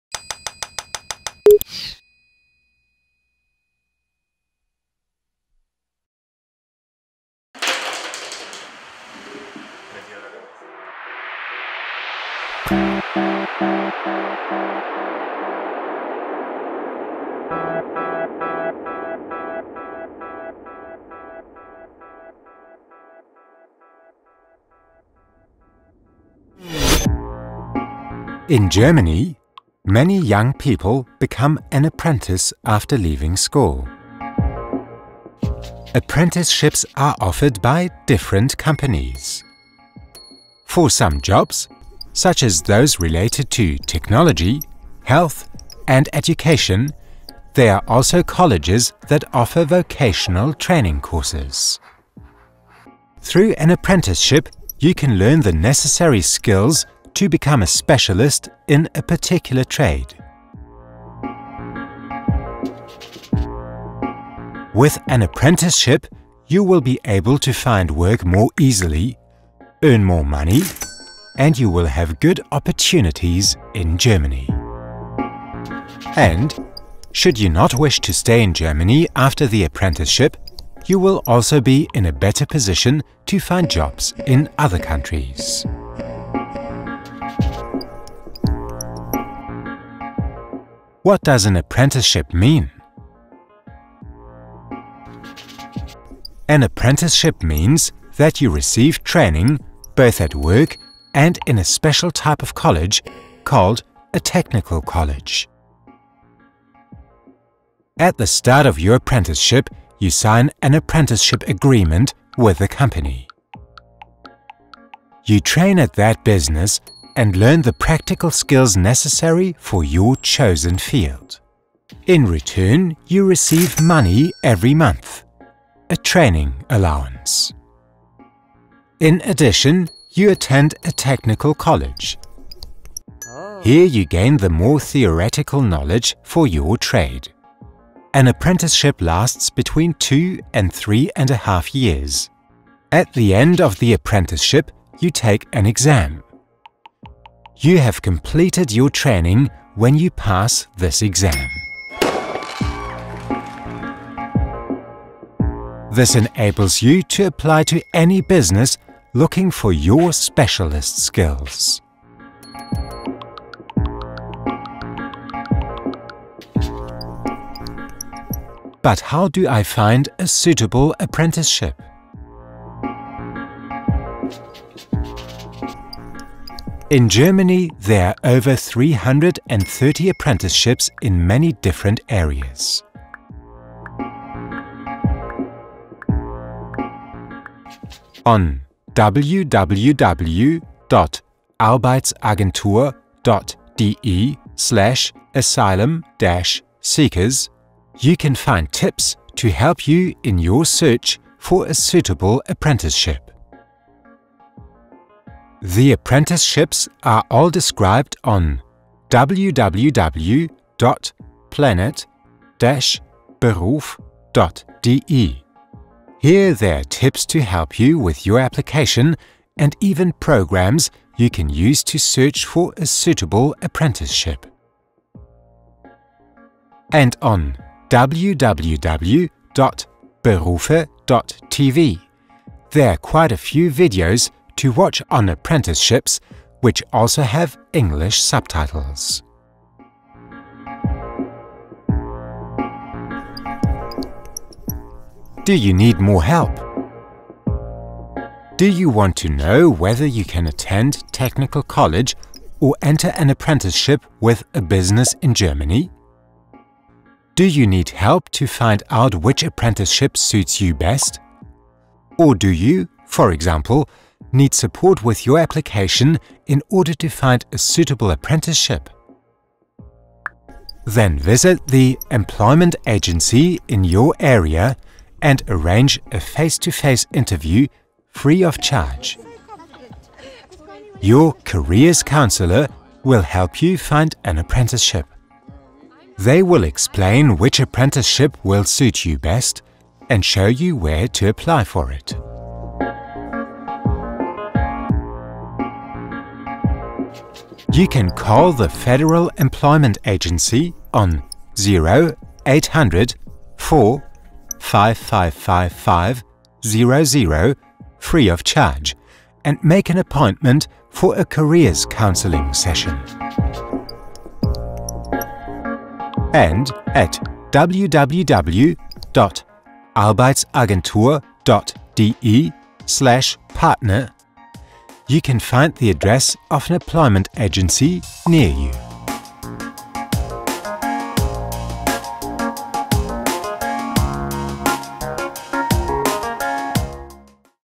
Apprenticeships in Germany
In Germany, many young people become an apprentice after leaving school.